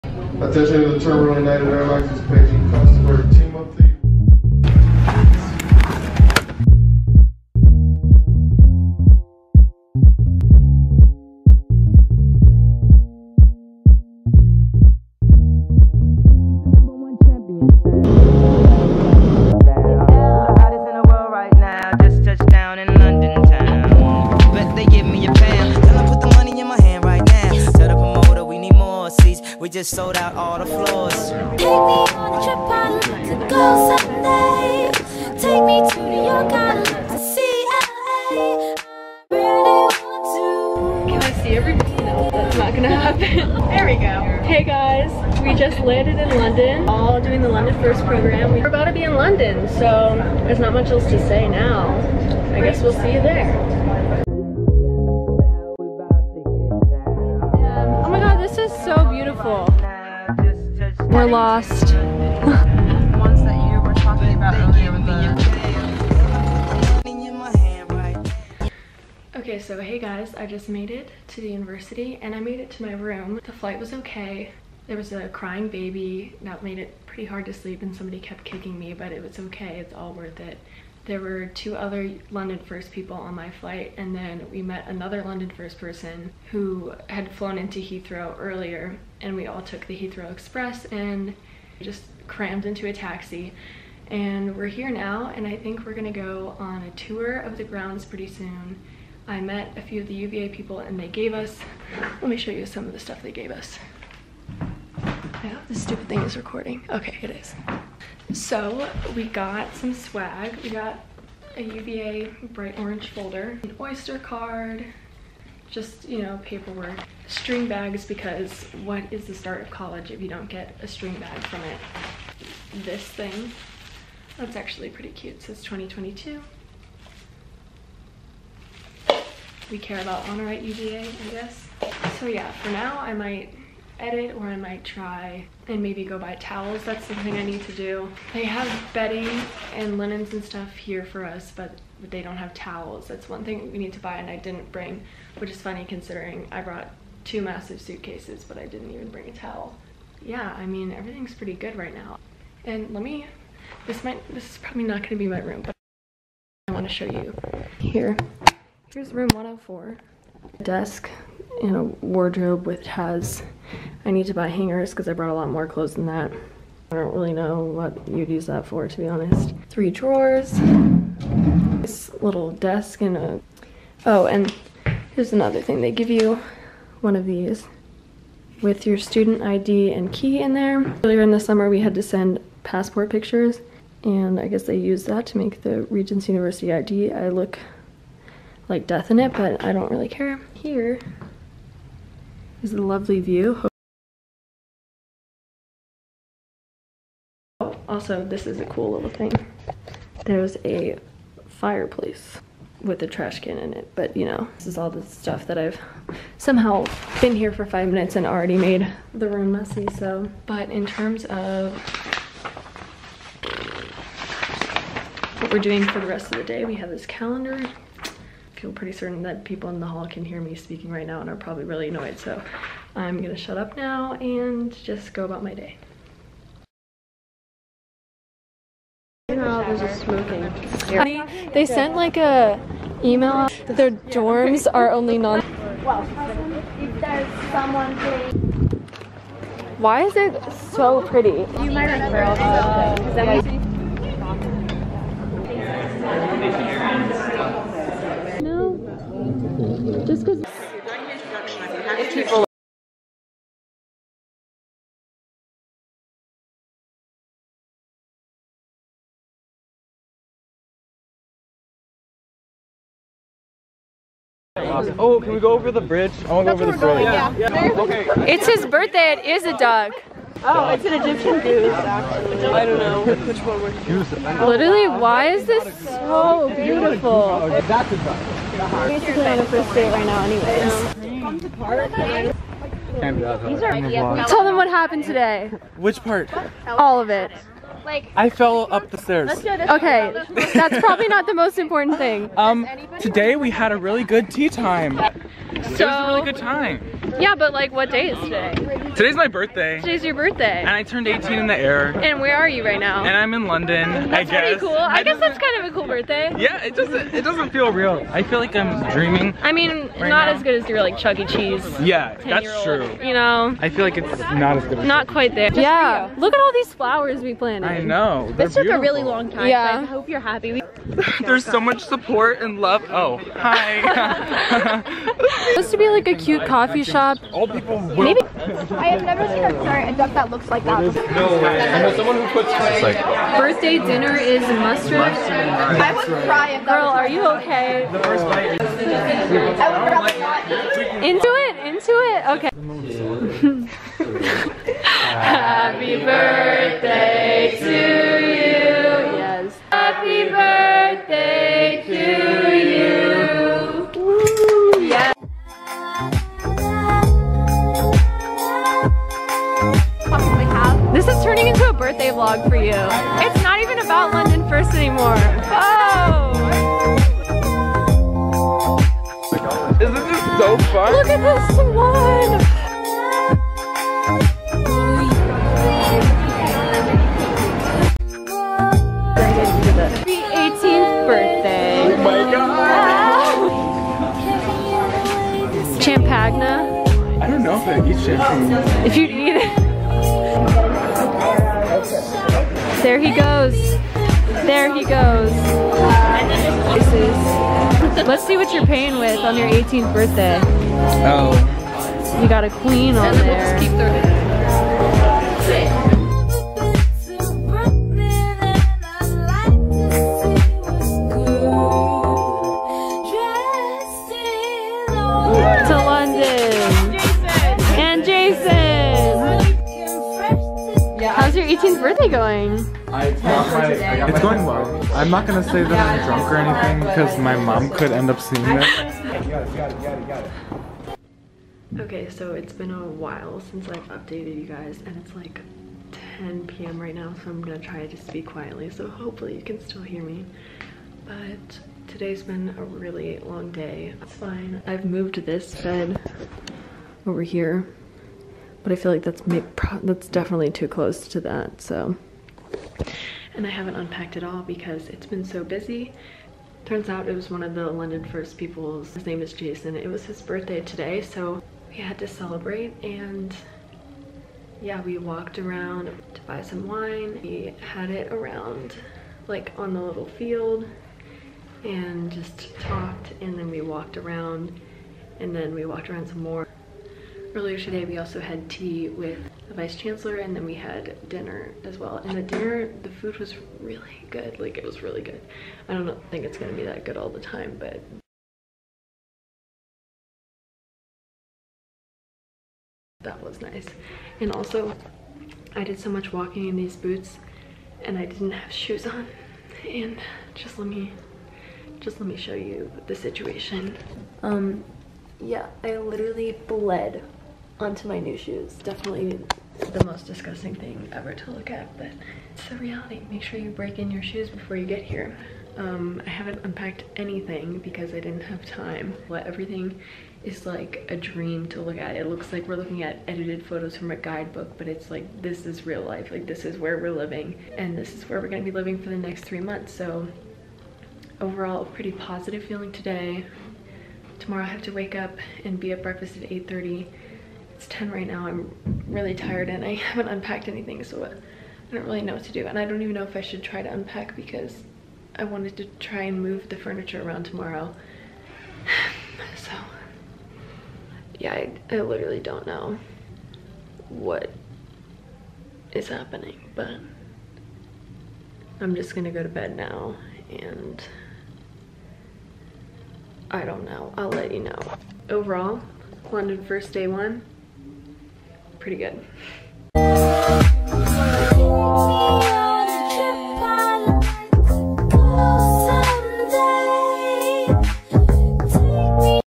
Moving to LONDON (UVA London First Travel Vlog)
hey guys! so FINALLY here is installment two (of many) of my vlogs of my first semester. I find it a little funny that I finally finished my video of my flight to London on my flight back to the US (sorry mom). with that being said, expect to see more consistent uploading now that I'm back and don't have a million things to see and do every day (as one does in London). in this vlog, I arrive in London, move in, and meet everyone in the program! it was also Jaison's birthday so of course we had to celebrate the British way.
I hope you guys enjoy and are excited to finally see what we got up to during this amazing semester!